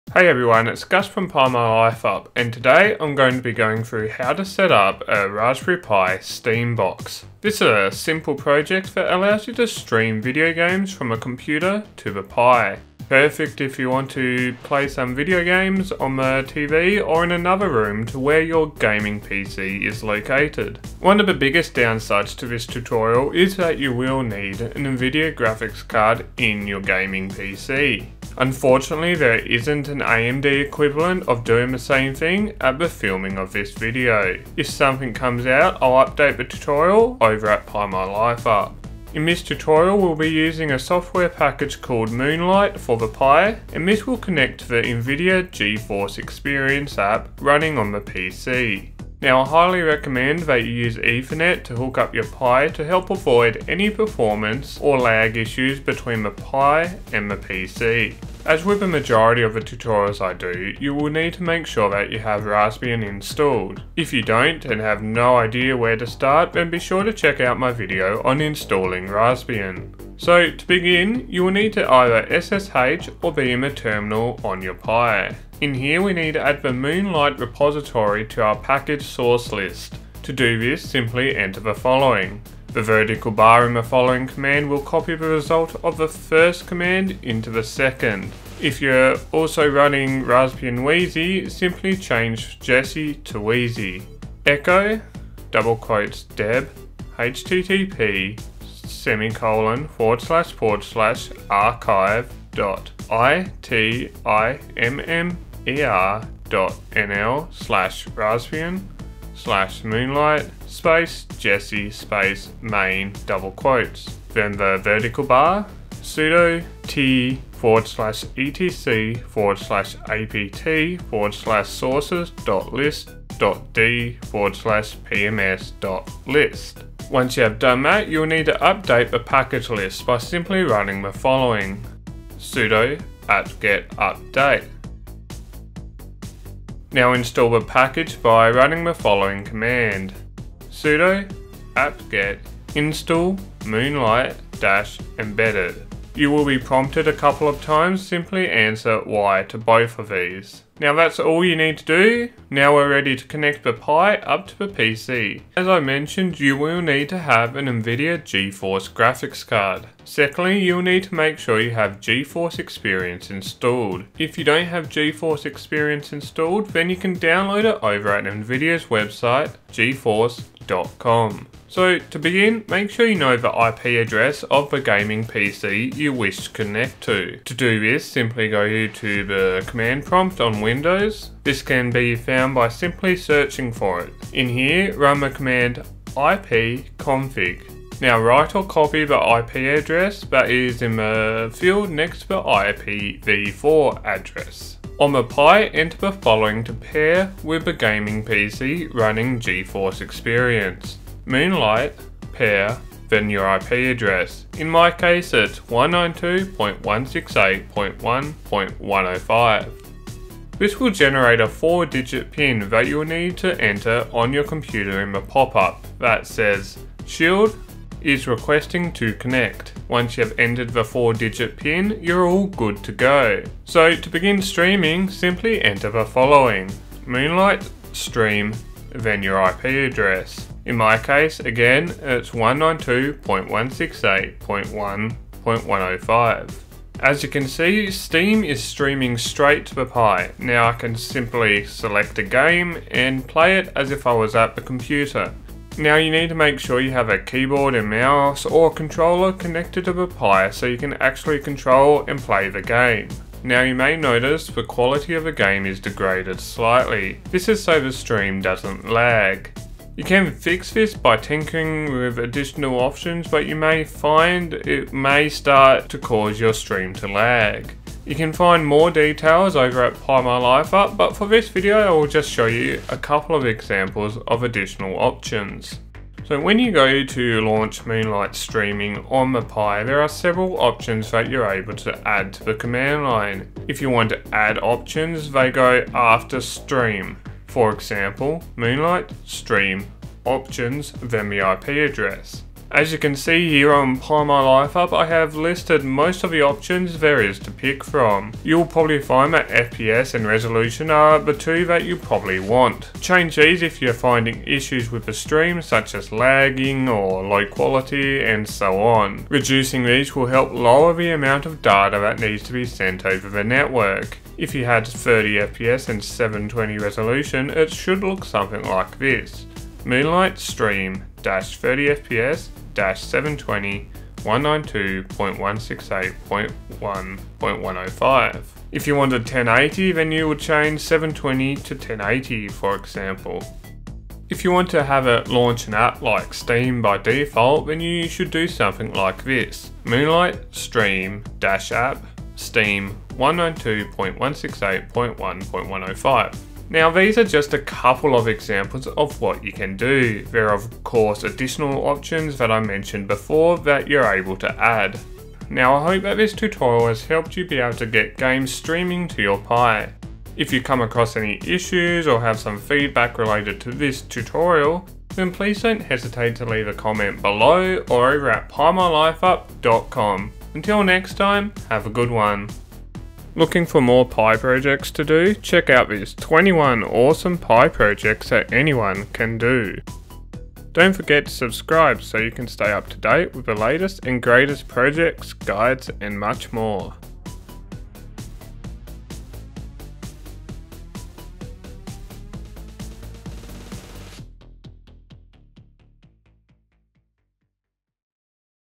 Raspberry Pi Nvidia Shield: Stream Games using Moonlight
In this video, I go through the steps to setting up Raspberry Pi Nvidia Shield streaming. This tutorial uses a software package called moonlight to connect to the Nvidia Geforce experience app. This means you will need a supported Nvidia Graphics card for it to work correctly.

Unfortunately, at the time of this video there wasn’t an AMD equivalent around. However, if one does pop up I’ll be sure to update the text version of this tutorial located over at Pi My Life Up.

This tutorial is pretty easy so if you’re a newbie to the Pi then you should have no trouble getting your head around this one. I do recommend that you use Ethernet as your network connection. You may notice a lot of lag trying to stream over WiFi.

Like almost every tutorial I do you will need to make sure you have Raspbian installed. This is very simple to do and you can find a video on how to do it on this channel.

There are quite a few extra options that you’re able to set for the Raspberry Pi Nvidia Shield streaming. These will help improve performance by sacrificing a bit of quality or vice versa.

I have tested quite a few games and found that most will actually not lose too much quality by streaming it. One game that I did notice a rather big difference was with Arma. However, with a bit of tinkering I am pretty sure you could get this to look as good as it did originally.

Now I hope this video has been able to help you get Raspberry Pi Nvidia Shield game streaming up and running.